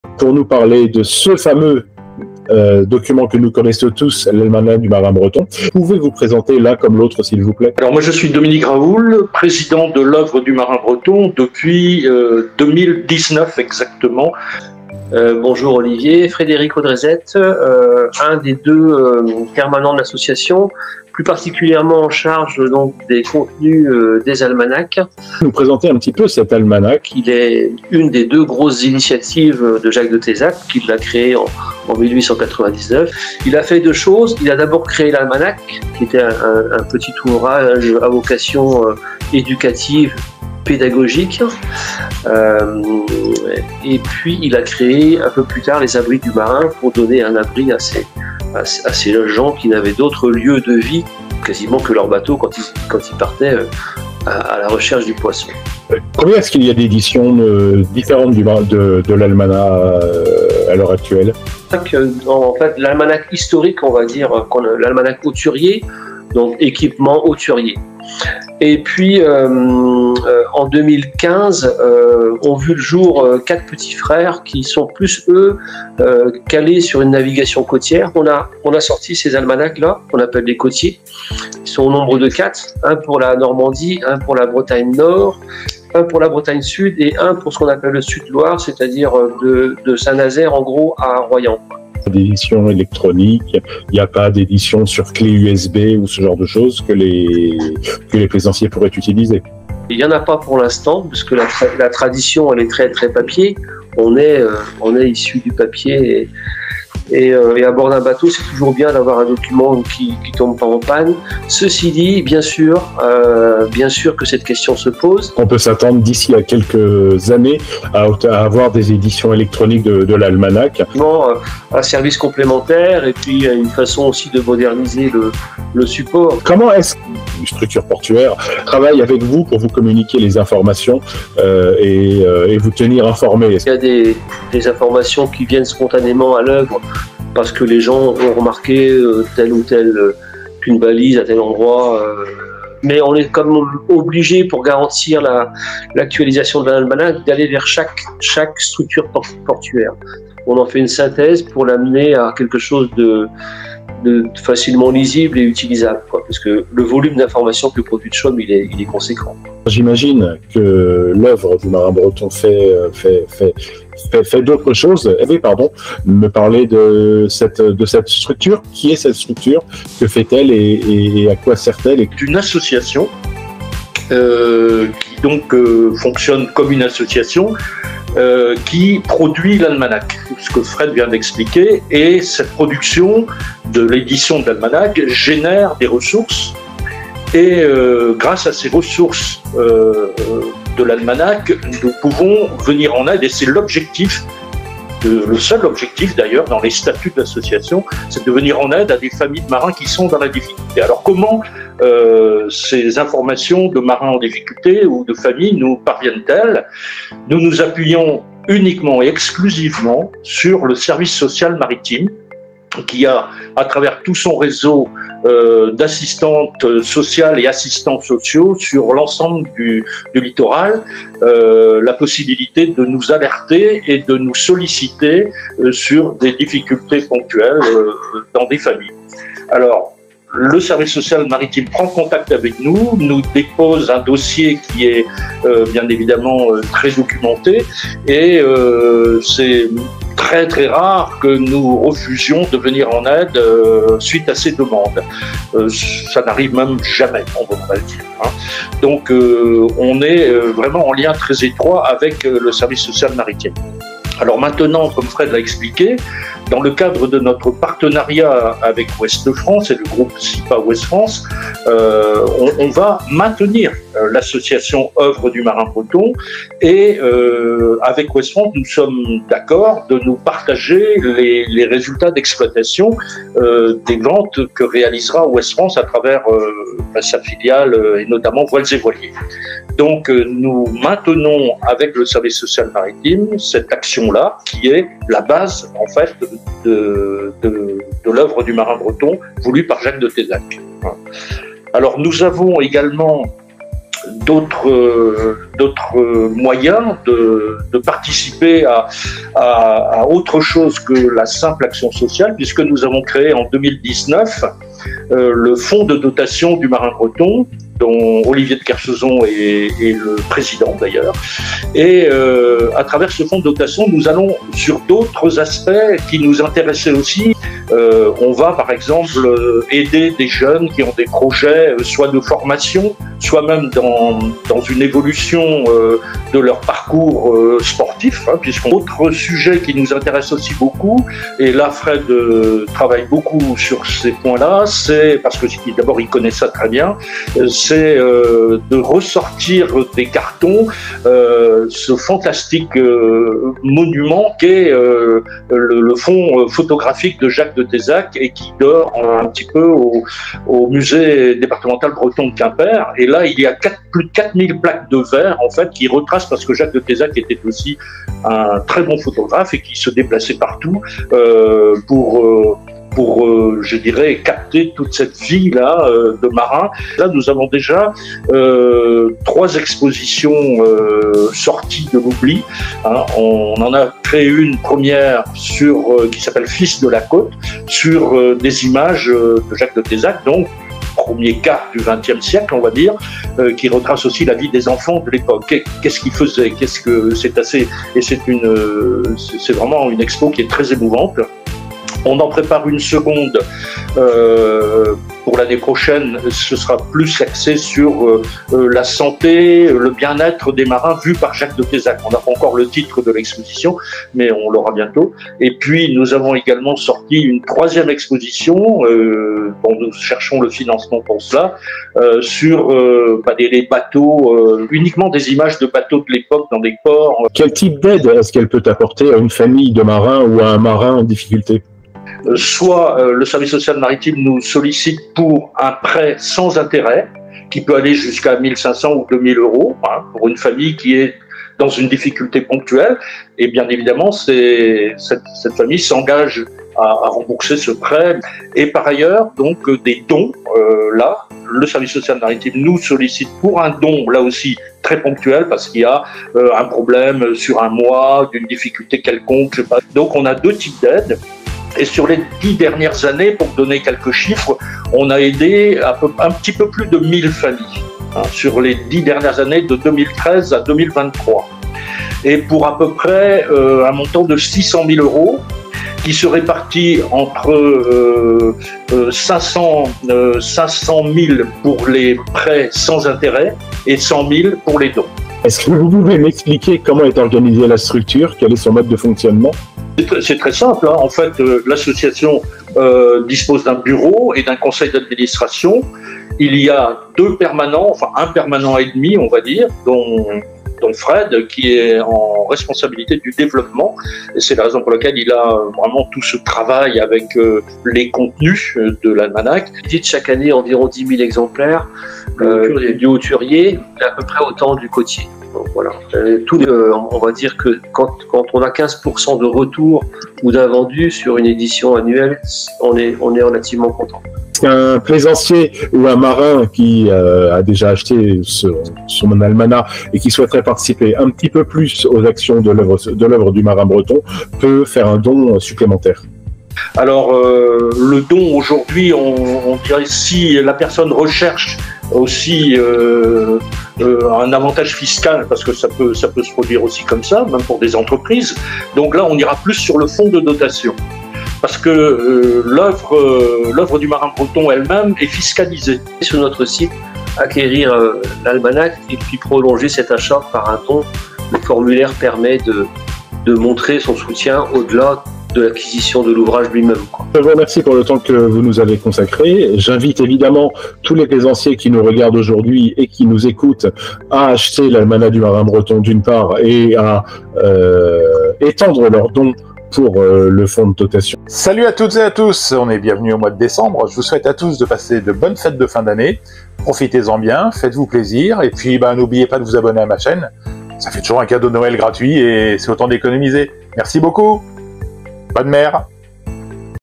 L'almanach du marin breton, bien plus qu'un simple document de bord !
Si l'almanach du marin breton reste la pièce maitresse de l'oeuvre entamée il y a plus d'un siècle par Jacques de Thezac, l'Oeuvre du Marin Breton se finance et propose son secours aux marins et à leurs familles grâce, notamment, aux ventes de ce document que tous les plaisanciers ont à leur bord.

L'almanach du Marin Breton est à la fois un condensé de règlementation, de cartographie et de culture maritime d'une inestimable valeur, édité chaque année par une association caritative trop peu connue.

Le financement des actions de l'association repose, évidemment, en grande partie sur ces ventes et, aussi, sur les dons des entrerprises comme des particuliers pour venir en secours aux gens de mer qui sont dans le besoin.

Découvrez l'arrière du décor, depuis la création de l'almanach du marin breton jusqu'aux aides que propose l'oeuvre créée en 1899 par Jacques de Thézac